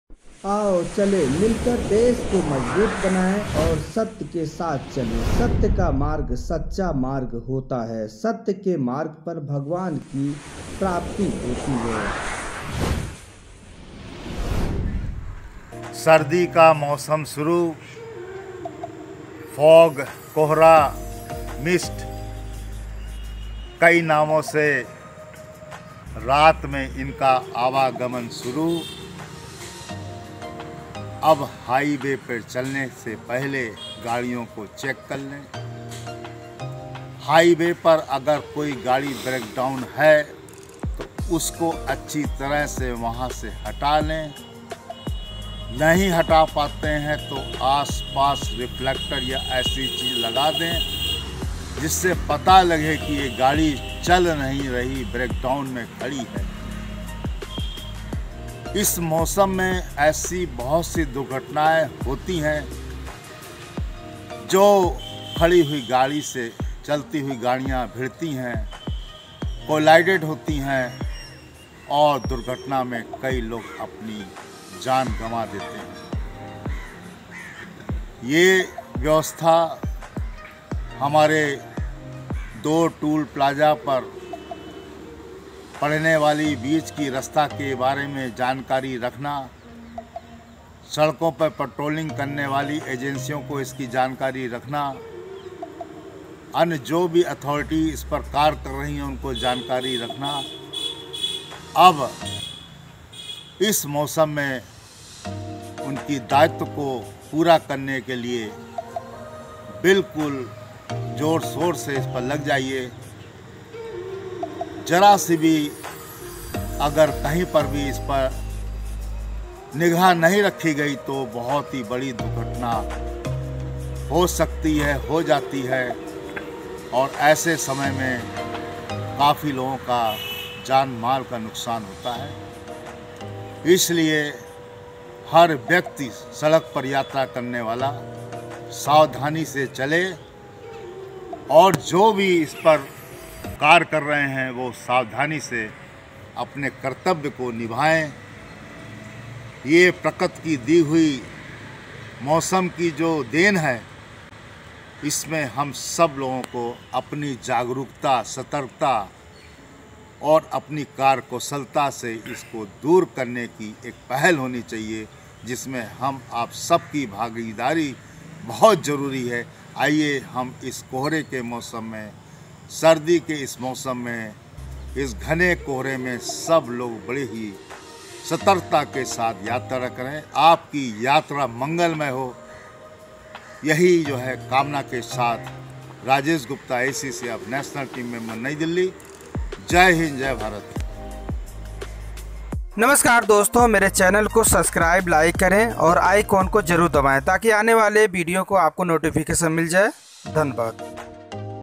आओ मिलकर मौसम का विशेष ख्याल करे सर्दी का मौसम शुरू कोहरा,फोग,मिस्ट आदि का आना शुरू हो चुकाहै सावधान
आओ मिलकर मौसम का विशेष ख्याल करे सर्दी का मौसम शुरू ओर कोहरा, फोग, मिस्ट आदि का आना शुरू ऐसे में सड़क यात्रा के समय बहुत सावधानी बरतने चाहिए, वेज़ व एक्सप्रेस वेज़ पर ब्रेकडाउन के वाहनों को  बिना अतिरिक्त सुरक्षा व्यवस्था के नही छोड़ना चाहिए अन्यथा जानमाल का नुकसान हो सकता है दुर्घटना भी हो सकती है सतर्कता बरतने के साथ यात्रा करनी चाहिए सुरक्षित पहुचना चाहिए आज सुबह टाइम्स टीम  नई दिल्ली